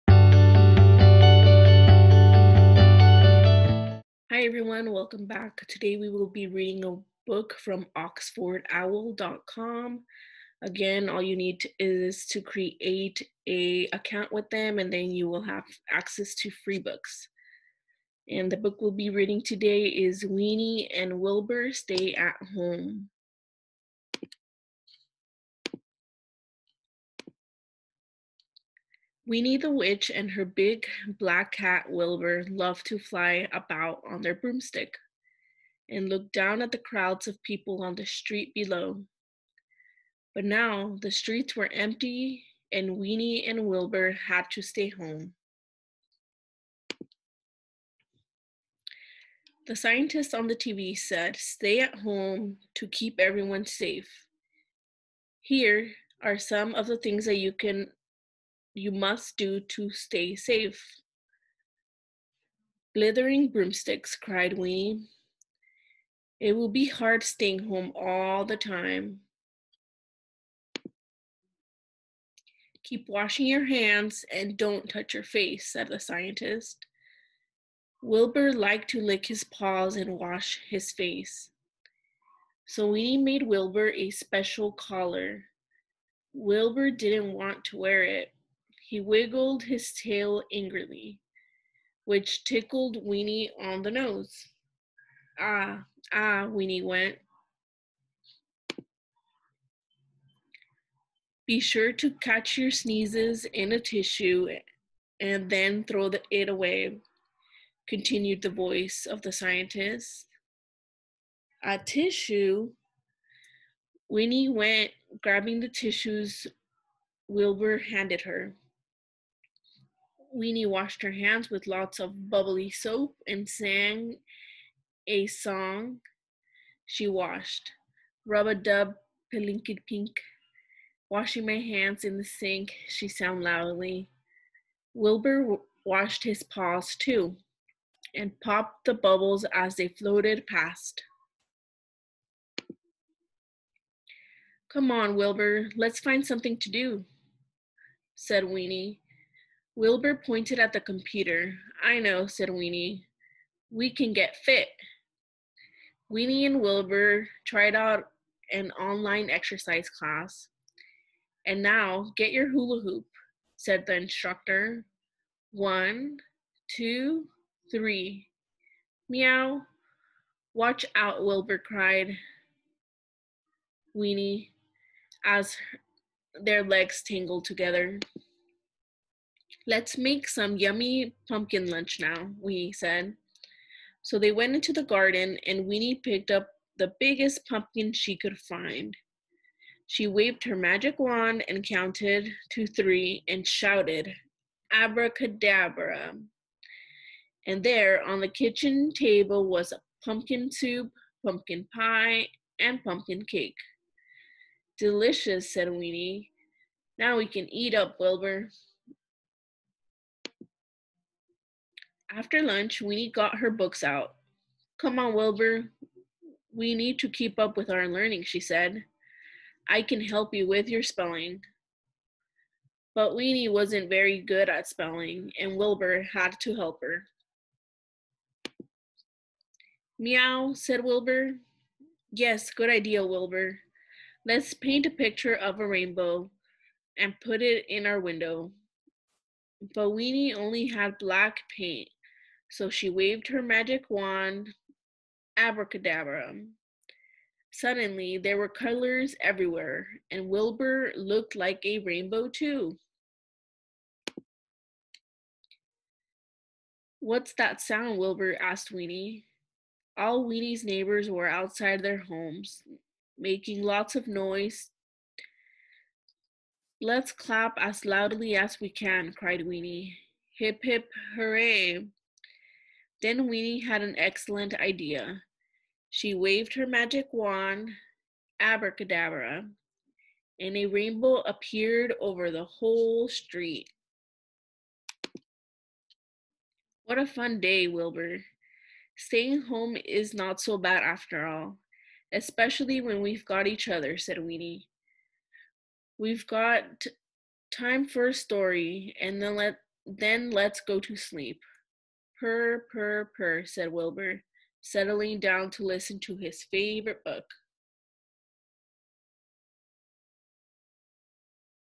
Winnie and Wilbur stay at home. Read Aloud by Janet
Can access website by going to Oxfordowl.com and creating an account.